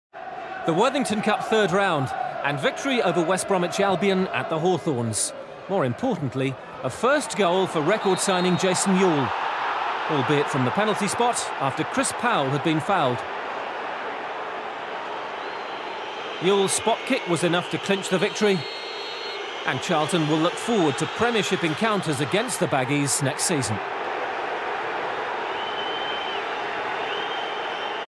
2001/02 WBA v Charlton Athletic LC 3rd Rd (Highlights)
9th October 2001 (Tues. – 7.45 p.m.) at The Hawthorns (17,734) - 3rd Round)
West Bromwich Albion 0 (0): Hoult; Lyttle (J. Chambers 71), Sigurdsson, Butler (Fox 46), Gilchrist, Clement; A. Chambers (Cummings 68), McInnes, Appleton; Dobie, Taylor. Subs. (not used) Jensen, Quinn.
Charlton Athletic 1 (1) (Euell 19 (pen)): Kiely; Robinson (Lisbie 86), Young, Brown, Fortune, Powell; Parker, Kinsella, Jensen (Konchesky 65); Euell, Johansson. Subs. (not used) Shields, Roberts, Peacock.
Referee: David Pugh (Wirral).
Booked: Robinson (78), time wasting; Johansson (80), scuffle with Sigurdsson; Sigurdsson (80), scuffle with Johansson.
Penalty awarded against Des Lyttle for tripping Chris Powell.
Sent-off: West Brom’s fitness instructor was ordered from the dugout following a scuffle to retrieve the ball from John Robinson.